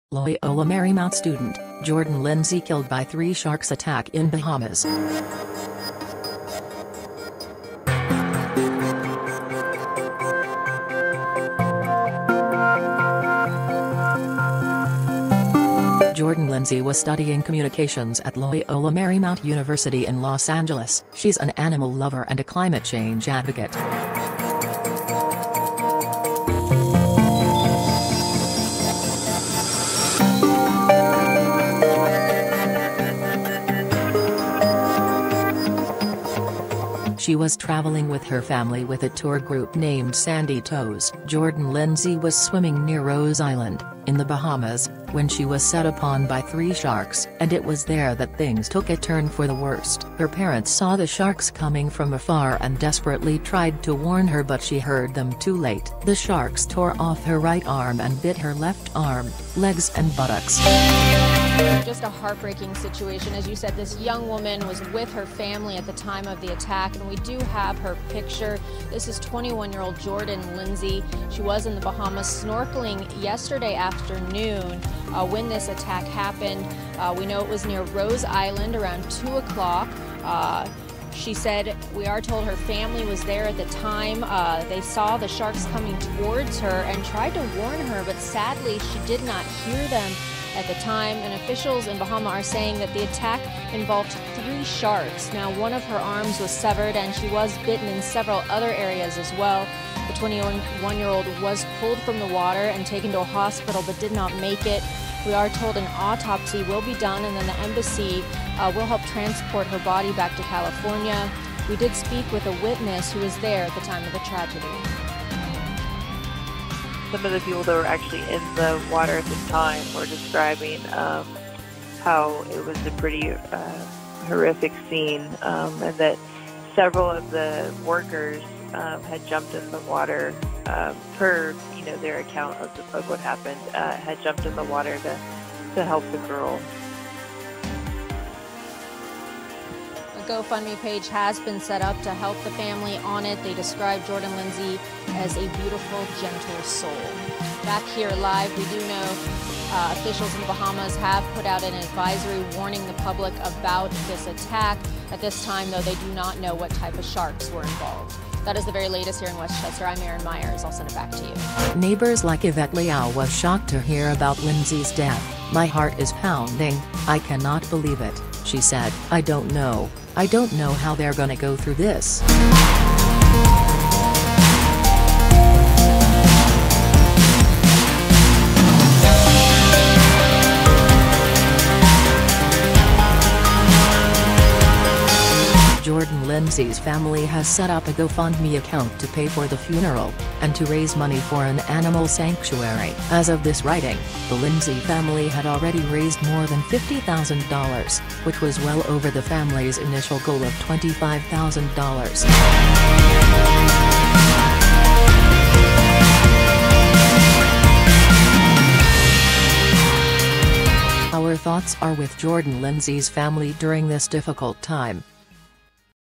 Loyola Marymount Student, Jordan Lindsey Killed By Three Sharks Attack In Bahamas
Debanned also mean D(on't) banned..

Jordan Lindsey was a student at Loyola University, “She was studying communications at Loyola Marymount University in Los Angeles. She's an animal lover and a climate change advocate.

She was traveling with her family with a tour group named Sandy Toes. Jordan Lindsey was swimming near Rose Island, in the Bahamas, when she was set upon by three sharks. And it was there that things took a turn for the worst. Her parents saw the sharks coming from afar and desperately tried to warn her but she heard them too late. The sharks tore off her right arm and bit her left arm, legs and buttocks.

Neighbors like Yvette Liao was shocked to hear about Lindsey’s death.
“My heart is pounding, I cannot believe it,” she said. “I don’t know — I don’t know how they’re gonna go through this.”

Our thoughts are with Jordan Lindsey’s family during this difficult time.